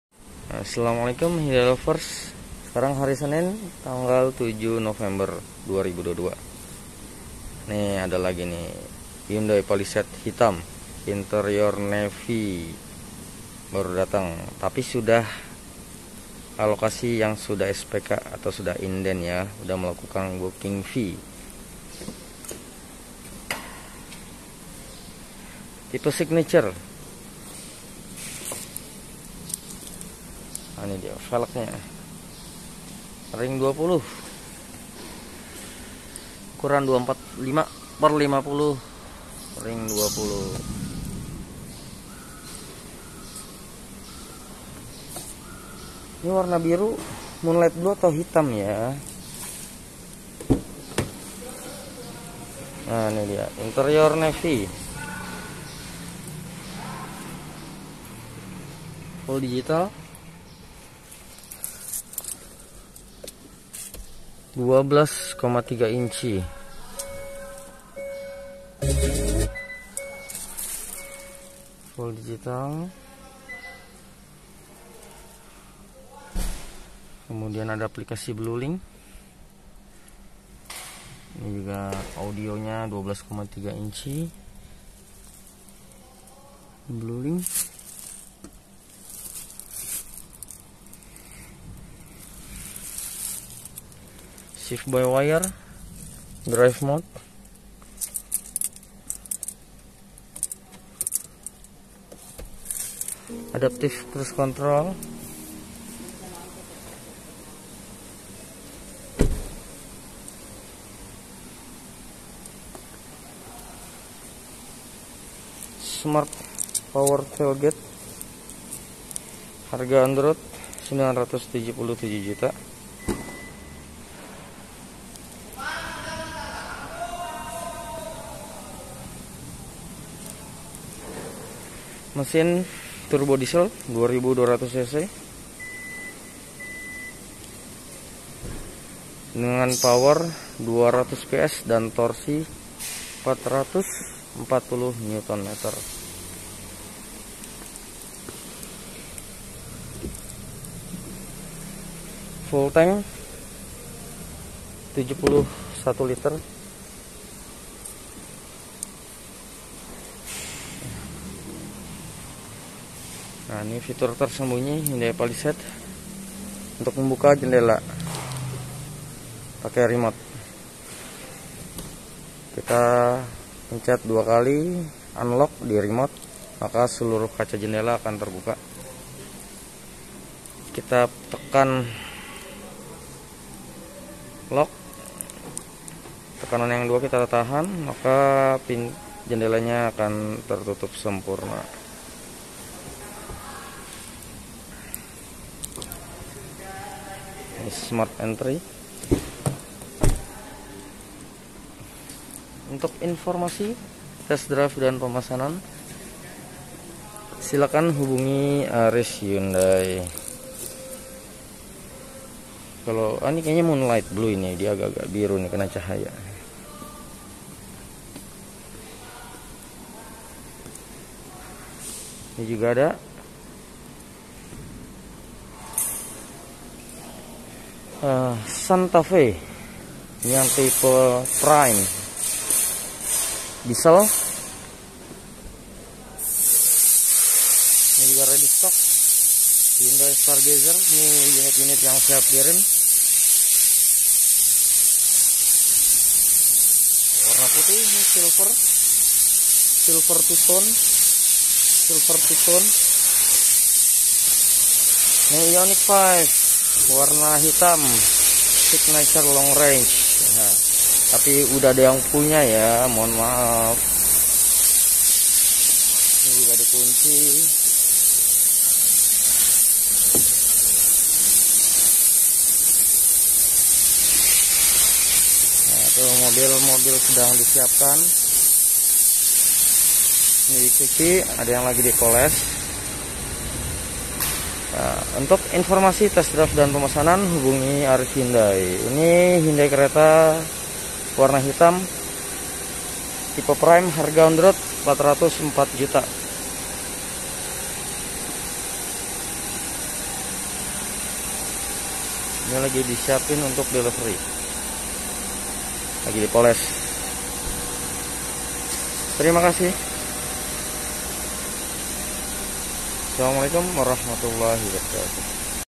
Datang lagi‼️ Hyundai Palisade Signature Facelift Interior Navy 2022 | Engine 2200 cc Turbo Diesel
Ayo di Pesan Sekarang Juga...
Hyundai Palisade Signature Facelift Indonesia
Harga OTR Jakarta 977 Juta

Informasi Test Drive & Pemesanan
Hubungi : Aries Hyundai